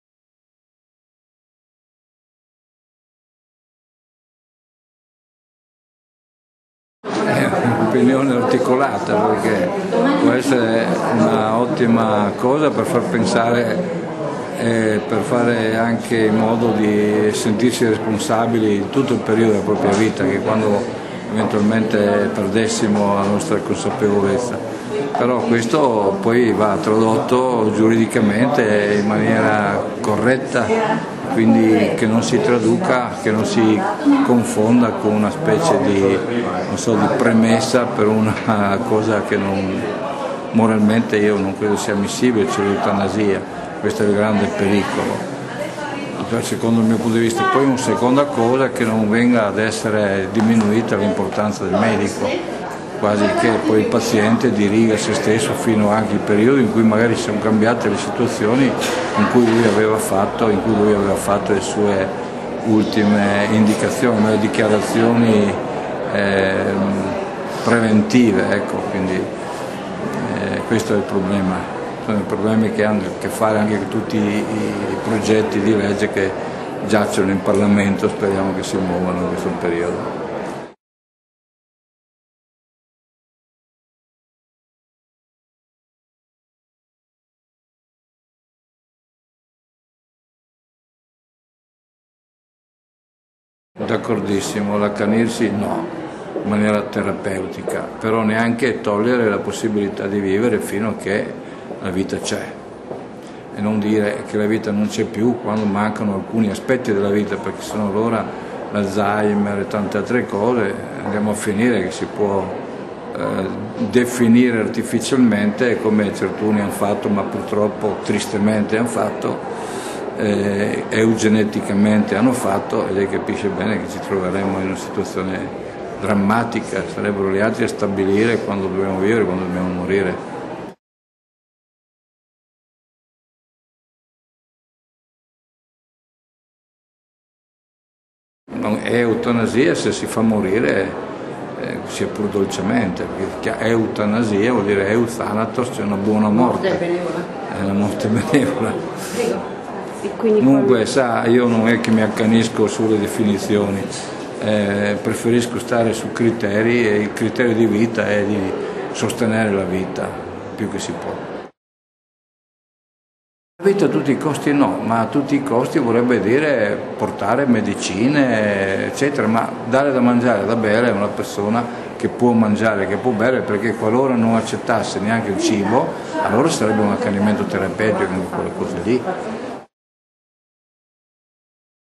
Finchè c'è vita c'è speranza...
Ma come si fa a capire quando la vita non c`&egrave; pi&ugrave;? Ne abbiamo parlato con Monsignor Luciano Padovese, uno dei protagonisti della tavola rotonda organizzata da FIDAPA, Lions Naonis e Soroptimist per parlare del testamento biologico. Guarda l`intervista e poi cerca quella al Dr. Marco Castelletto nella categoria "Questa settimana".